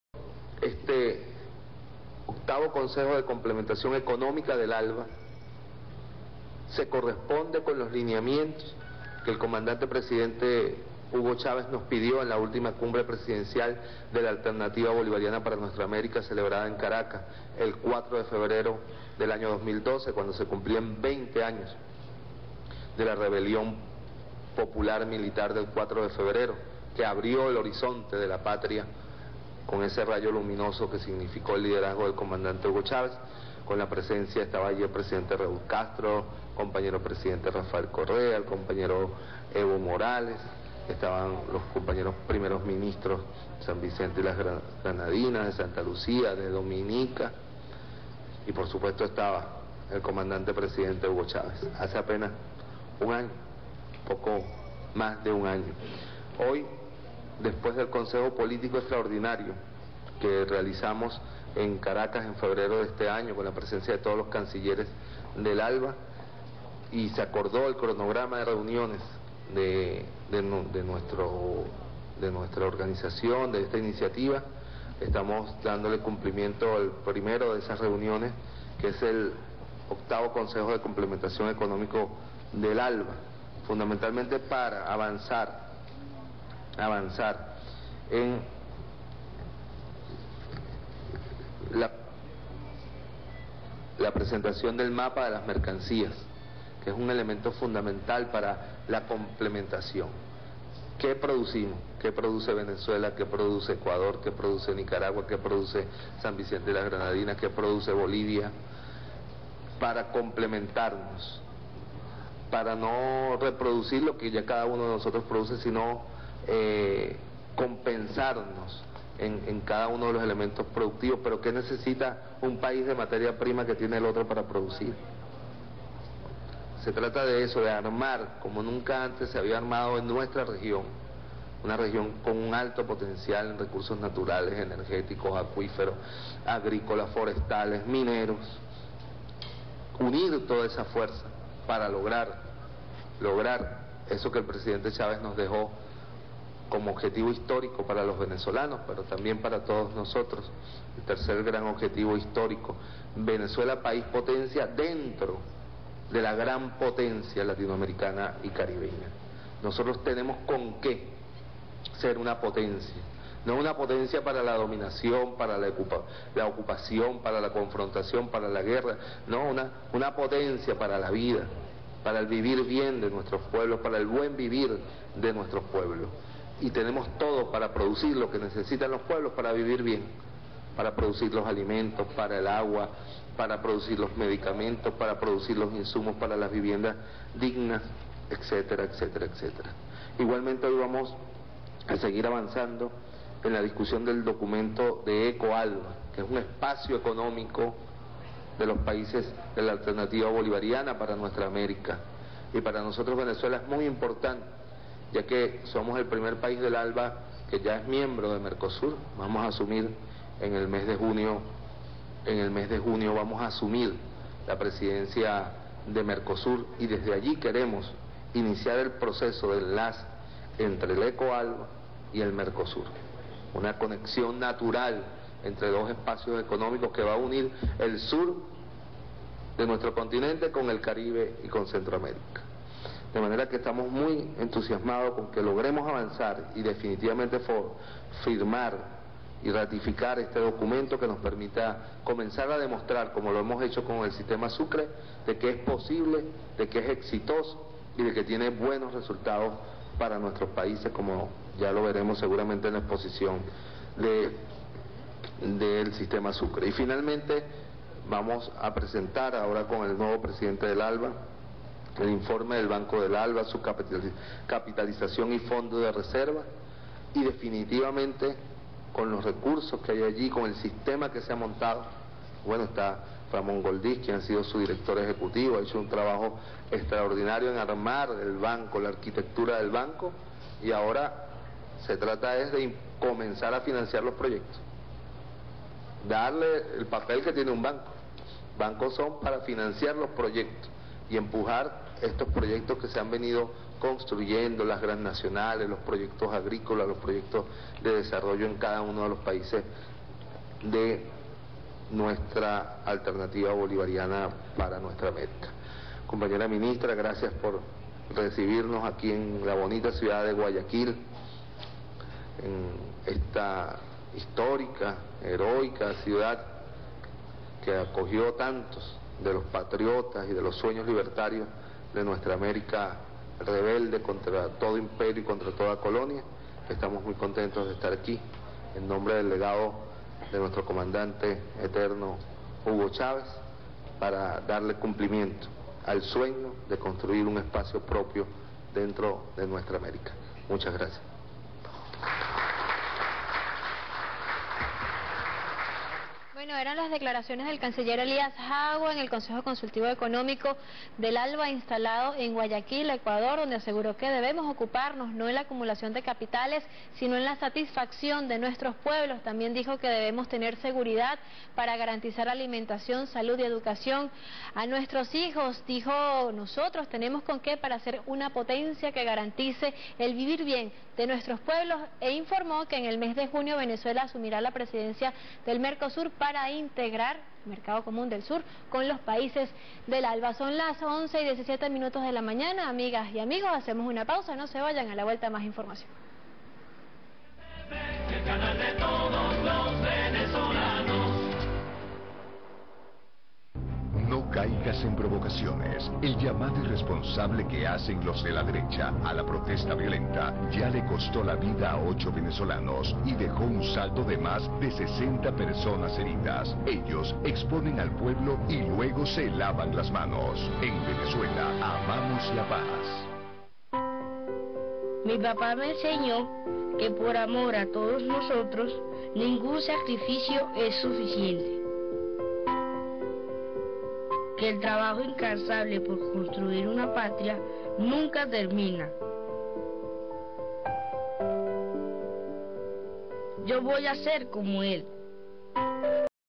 "Nosotros tenemos con qué ser una potencia, una potencia para la vida": Jaua desde Ecuador
El canciller de la República, Elías Jaua, participó este martes en un consejo económico del Alba en Ecuador.